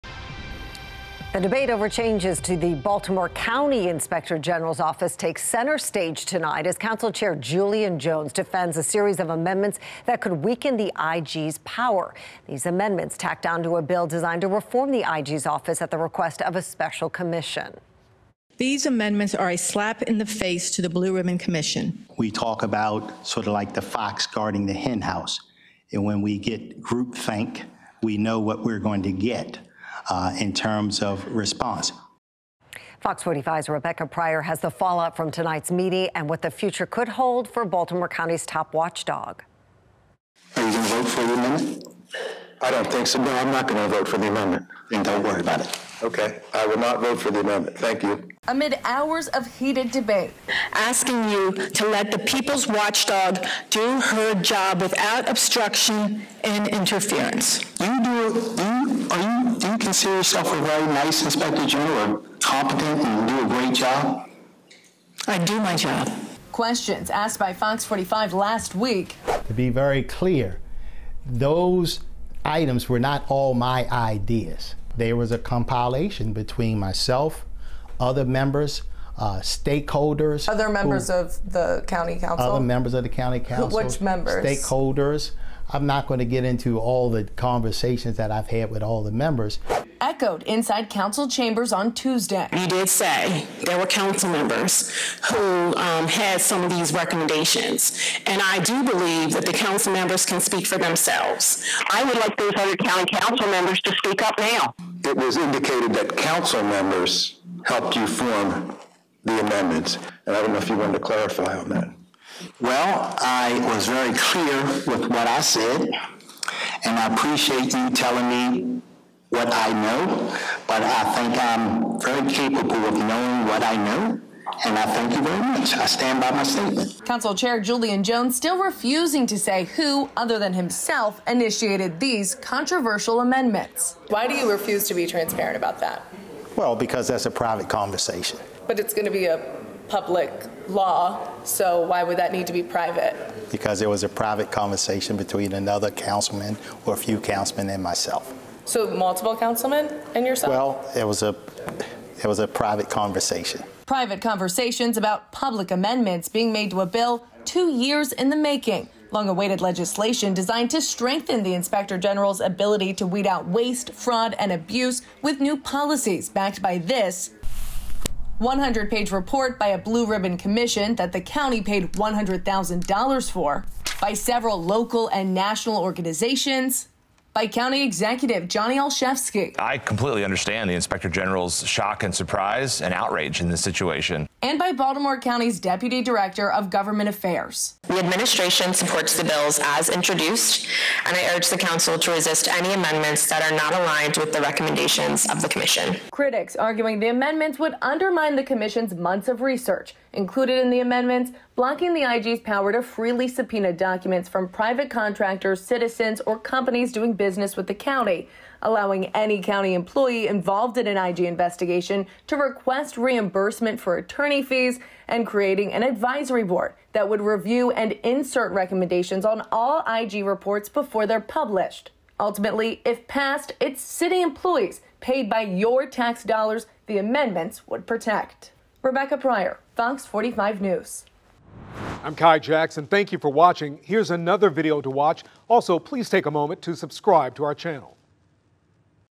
Baltimore Council Chair heavily criticized for trying to limit Inspector General's power
Baltimore County's council chambers were flooded with hours of heated debate Tuesday night as Council Chair Julian Jones faced sharp criticism from residents, local leaders, and other council members concerning his efforts to weaken the authority of the Baltimore County Inspector General.

The controversy began last week when Jones introduced a slew of last-minute amendments to a bill designed to strengthen the IG's ability to weed out waste, fraud, and abuse.